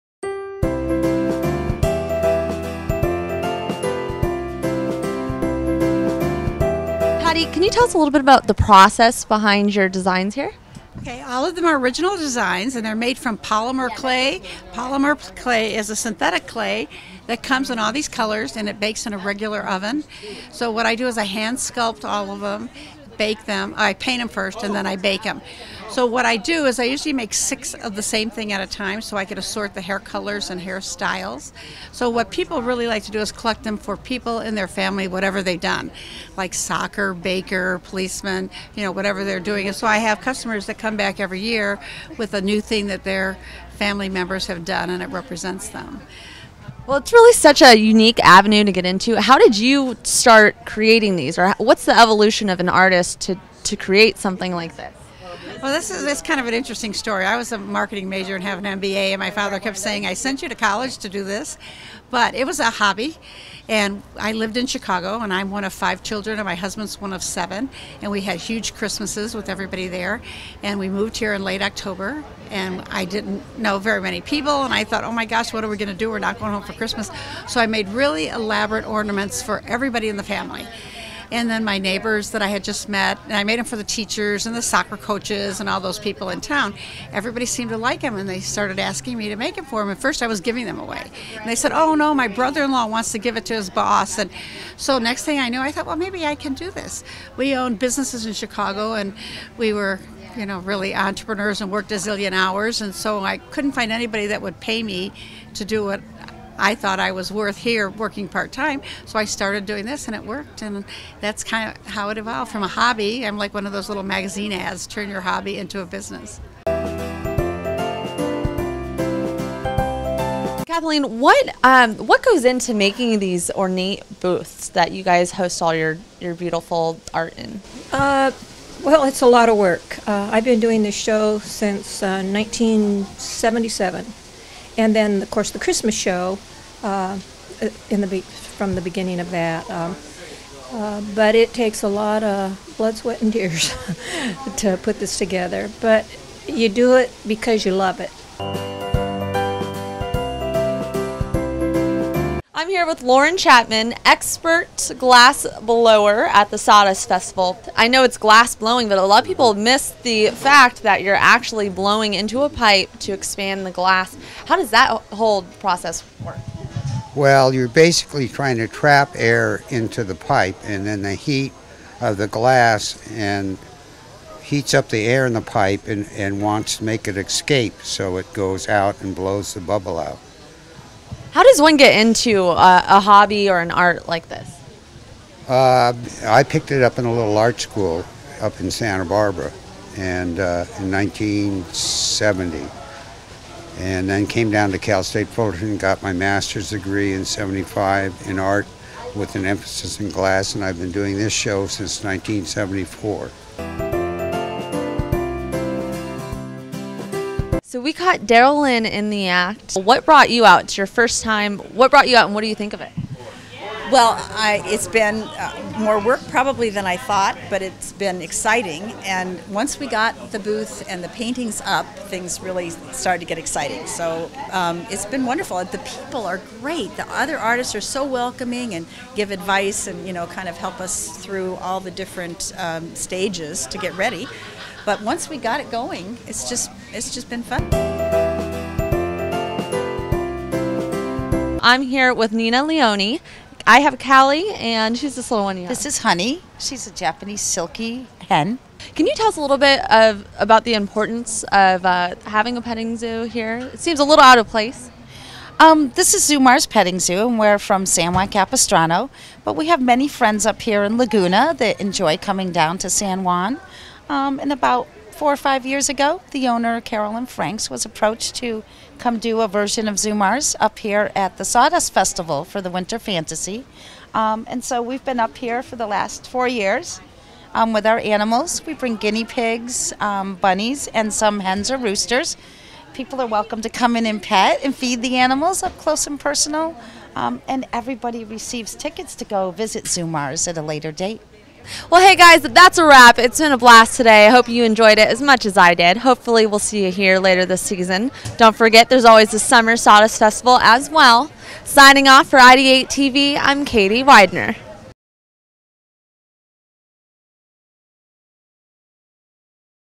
IdeateTV gets inspired by the artists - Winter Fantasy Sawdust Festival 2013
IdeateTV host Katie Widner visits the Winter Fantasy Sawdust Festival 2013. She speaks with several of the artists to learn about their passion for the arts and the significance of being at the Sawdust Festival.